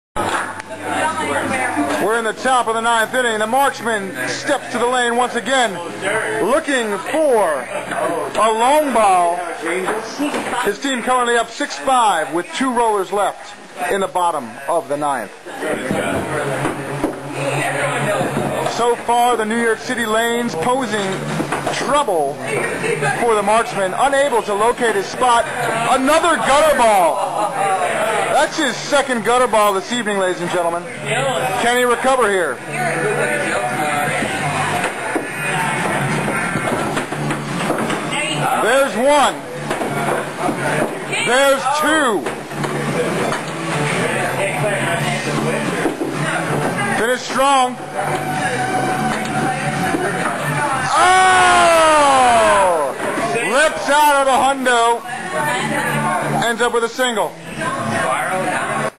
Marksman Looks For Long Ball in The 9th
Lips out of The Hundo, ends up with a single. I bet nobody has ever said that before.